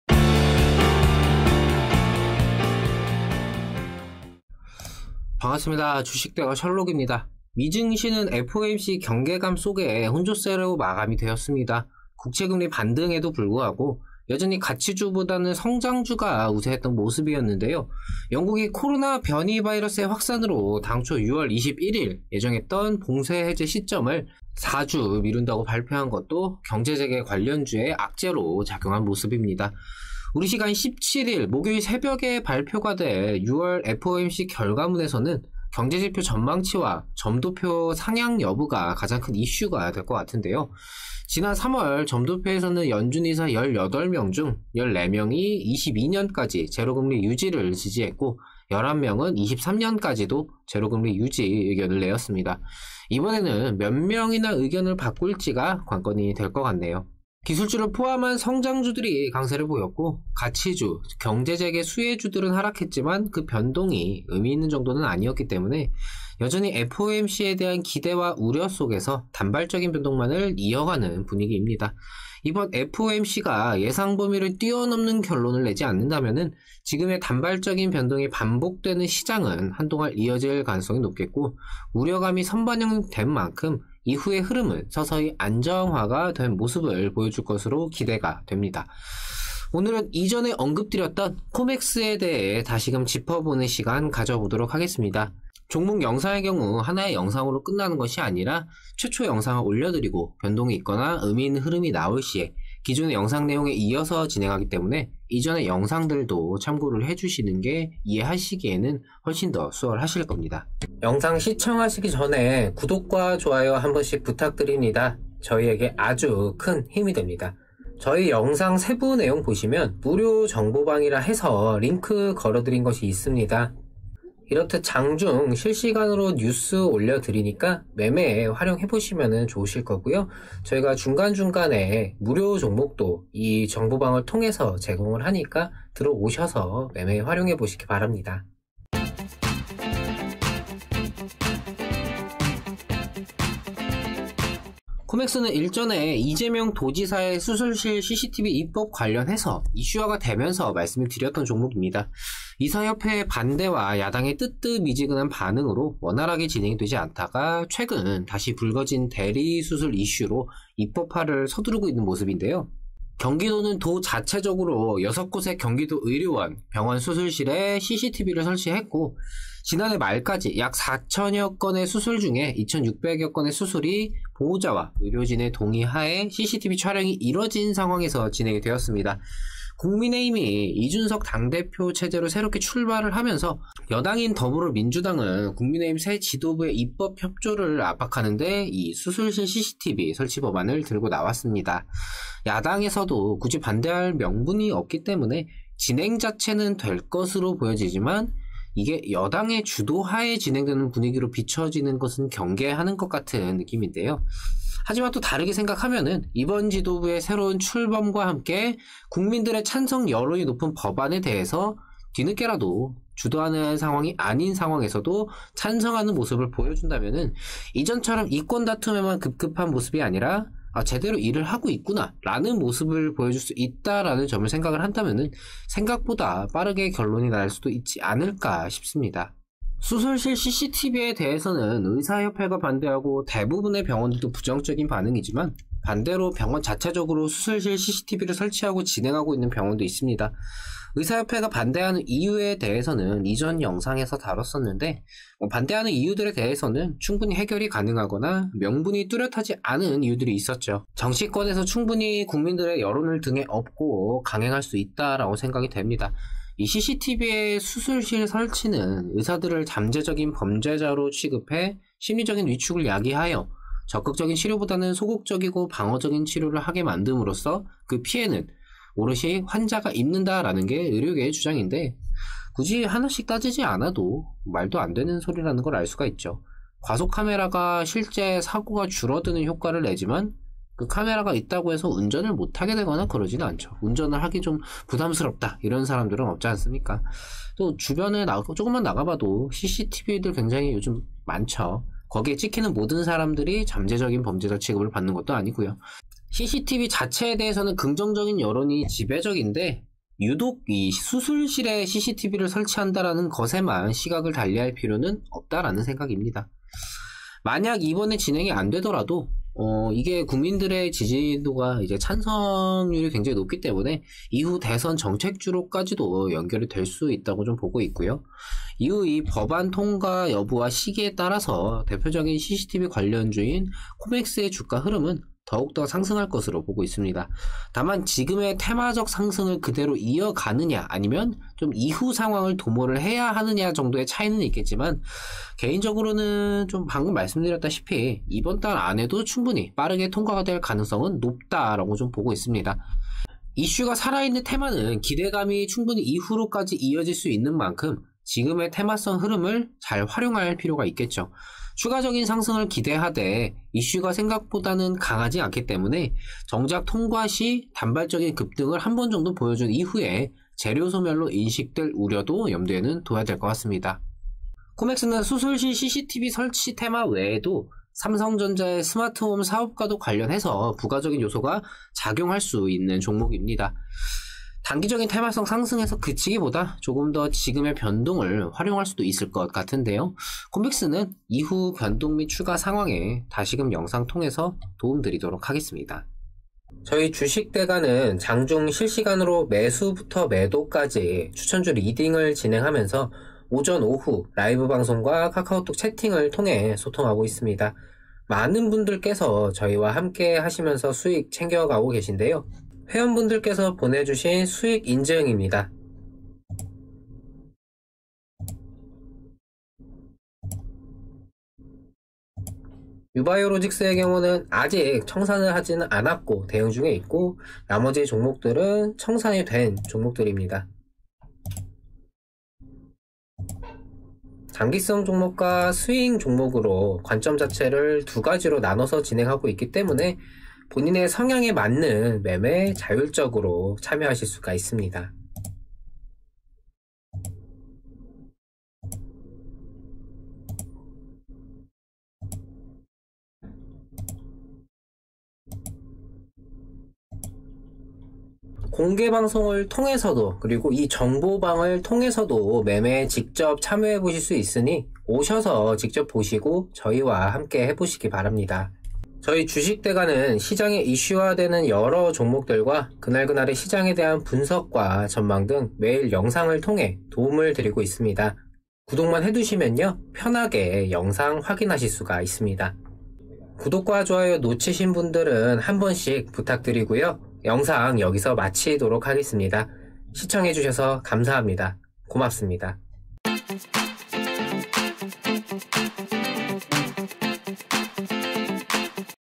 코맥스 - 수술실 CCTV 설치 법안 의무화 삼성과의 스마트홈 사업까지?!
수술실 CCTV 설치 이슈와 삼성전자와의 스마트홈 협업 관련 기대감이 작용할 수 있는 코맥스에 대해 짚어보았습니다

방송시간  ★오전8시 45분 ★오후 2시 50분